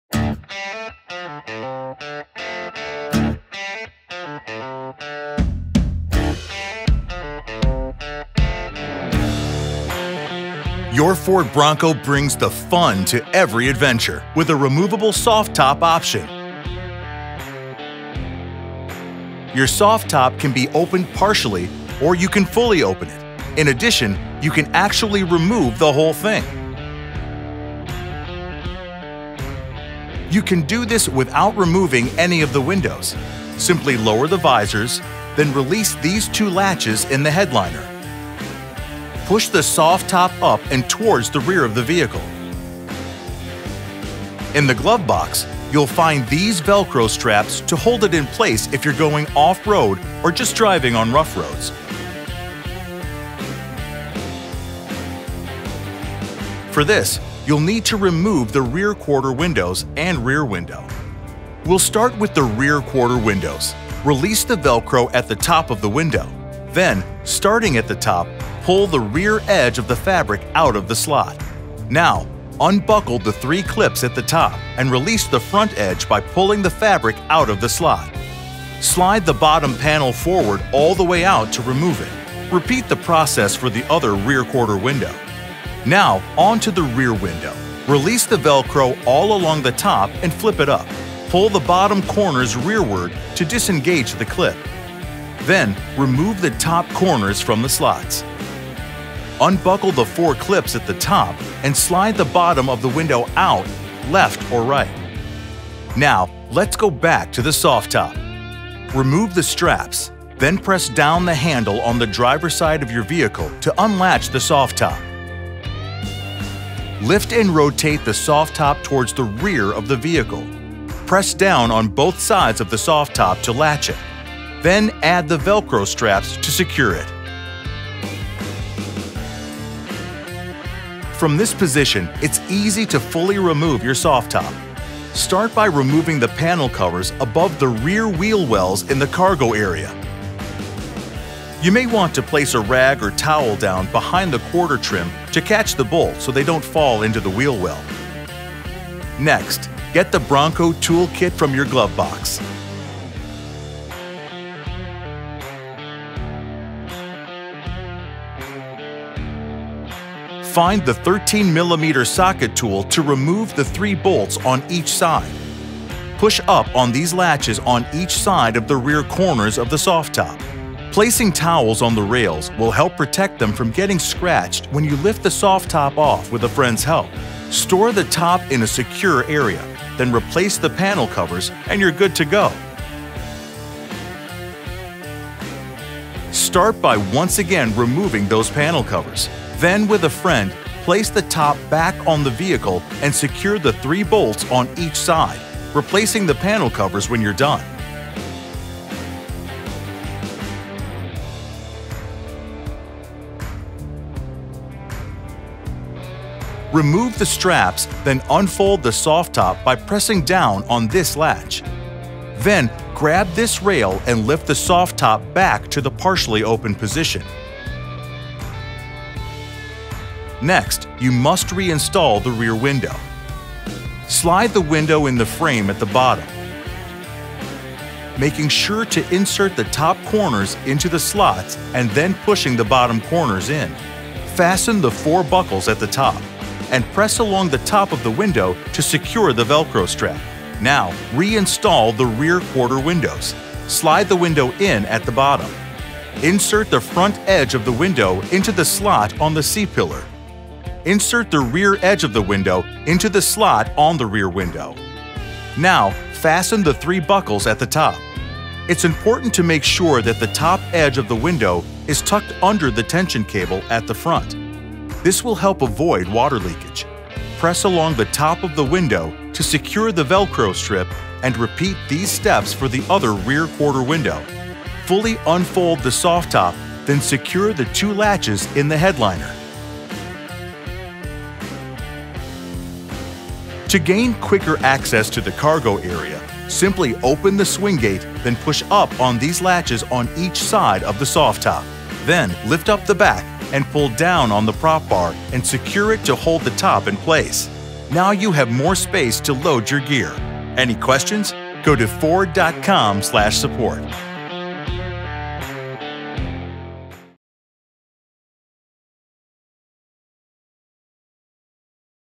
Ford Bronco™ Soft Top Removal and Installation | Ford How-To
Learn how to remove and reinstall the soft top on your Ford Bronco™ SUV with this easy-to-follow video.

Preproduction vehicles shown with optional equipment and available Ford Accessories. Vehicle available beginning summer 2021.